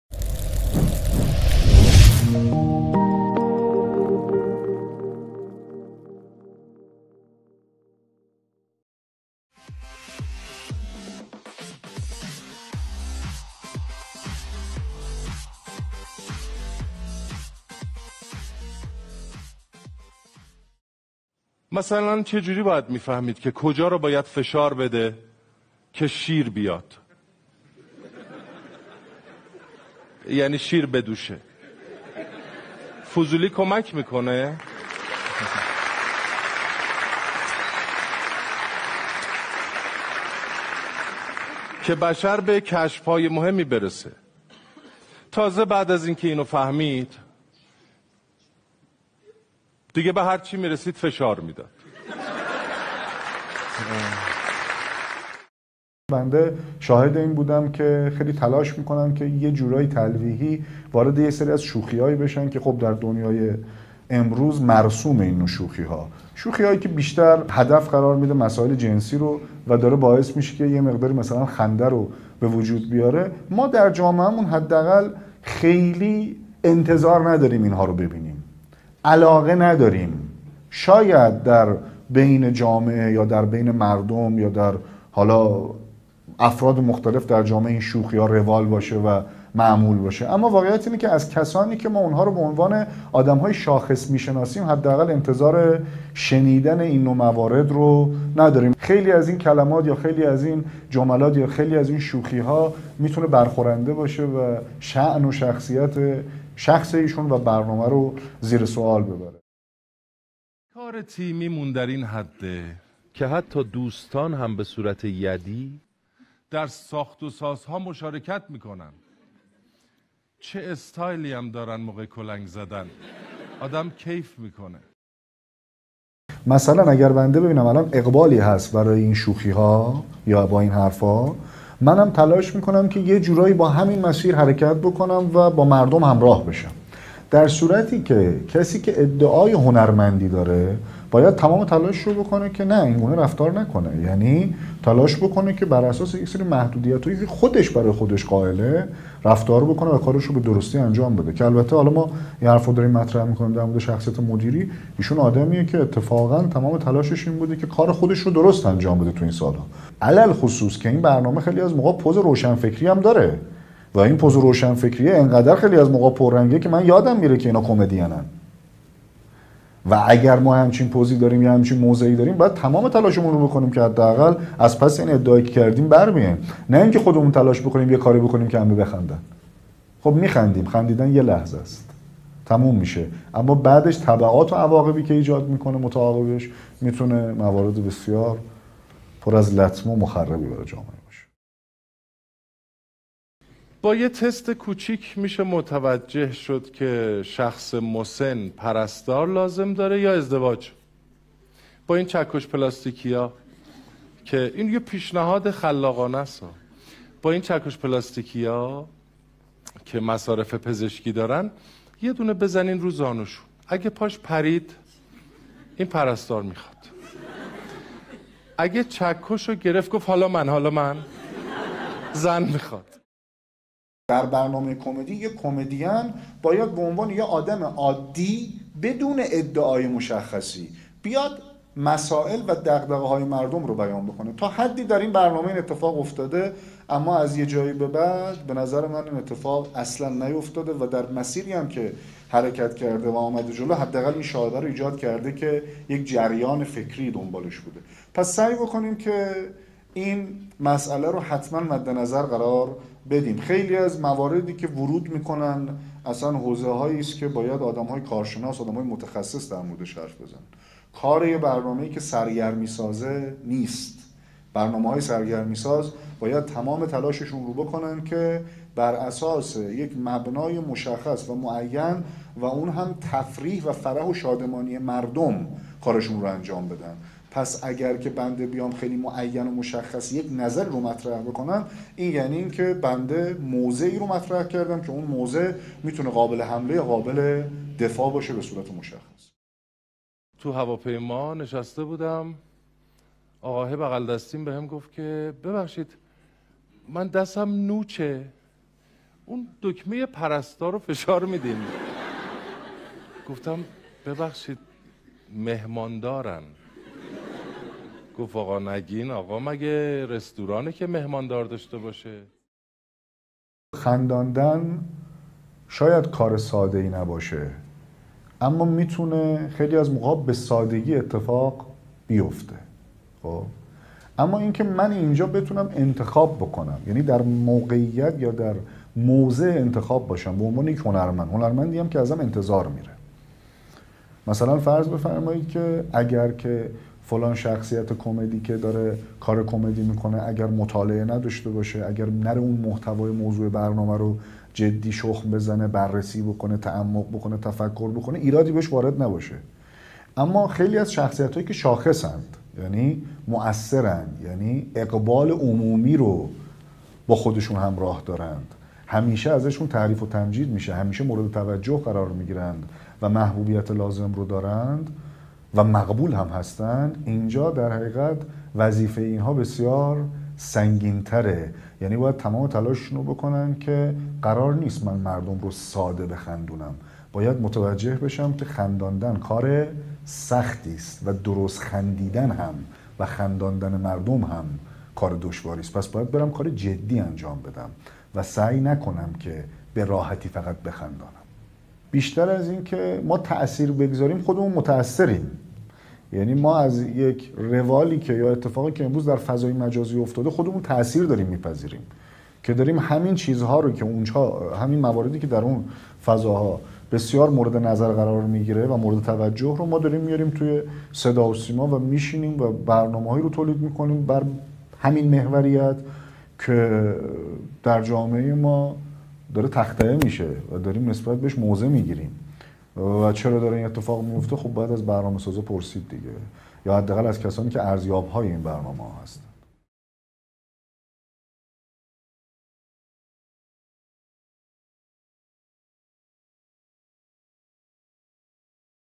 پایان قسمت سوم؛ ضعف و قوت برنامه دورهمی؛تاثیرگذاری در جامعه و نقش مهران مدیری + فیلم
سومین نشست نقد برنامه دورهمی و مهران مدیری به پایان رسید.